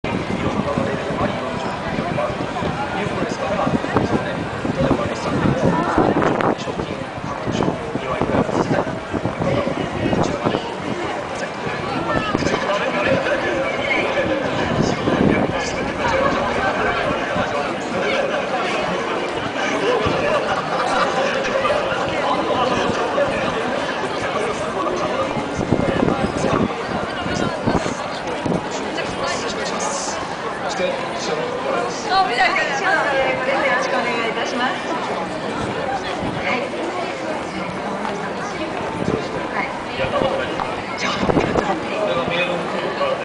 Osaka : El Cartel de Glico
El cartel de glico se ha convertido en uno de los simbolos de Osaka, Japon.